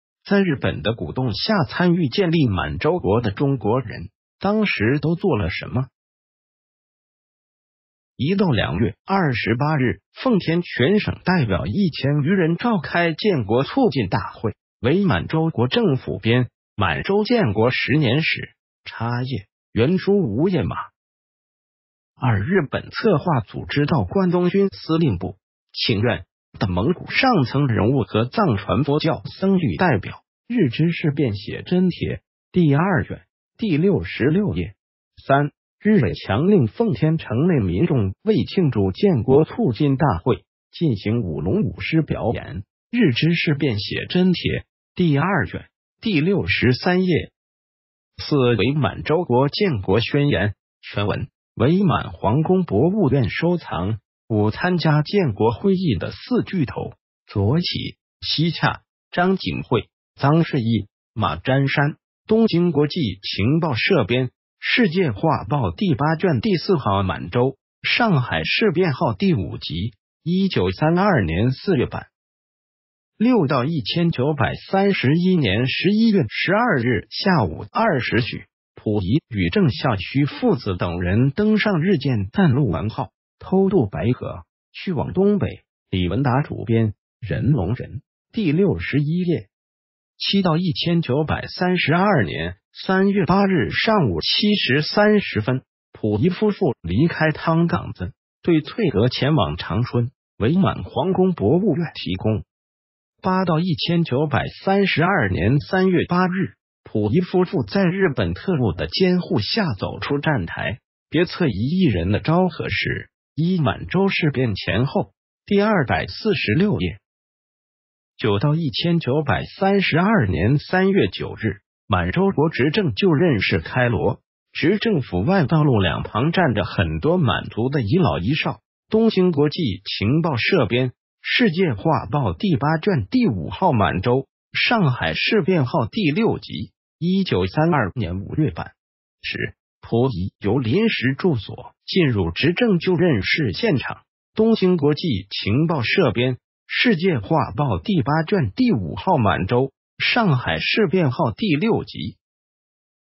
在日本的鼓动下参与建立“满洲国”的中国人，当时都做了什么？
1-2月28日奉天全省代表1000余人召开“建国促进大会”。〔伪满洲国政府编：《满洲建国十年史》，插页，原书无页码〕

2-日本策划组织到关东军司令部“请愿”的蒙古上层人物和藏传佛教僧侣代表。〔《日支事变写真帖》第二卷，第66页〕

3-日伪强令奉天城内民众为庆祝“建国促进大会”进行舞龙舞狮表演。〔《日支事变写真帖》第二卷，第63页〕

4-伪满洲国“建国宣言”全文。〔伪满皇宫博物院收藏〕

5-参加“建国会议”的“四巨头”，左起：熙洽、张景惠、臧式毅、马占山。〔东京国际情报社编：《世界画报》第8卷第4号《满洲、上海事变号》第五辑，1932年4月版〕

6－1931年11月12日下午2时许，溥仪与郑孝胥父子等人登上日舰“淡路丸”号，偷渡白河，去往东北。〔李文达主编：《人龙人》，第61页〕

7-1932年3月8日上午7时30分，溥仪夫妇离开汤岗子对翠阁前往长春。〔伪满皇宫博物院提供〕

8-1932年3月8日，溥仪夫妇在日本特务的“监护”下走出站台。〔《别册一亿人的昭和史（1）·满洲事变前后》，第246页〕

9-1932年3月9日，“满洲国执政就任式”开锣，“执政府”外道路两旁站着很多满族的遗老遗少。〔东京国际情报社编：《世界画报》第8卷第5号《满洲、上海事变号》第六辑，1932年5月版〕

10-溥仪由临时住所进入“执政就任式”现场。〔东京国际情报社编：《世界画报》第8卷第5号《满洲、上海事变号》第六辑〕
编辑 |别毕卉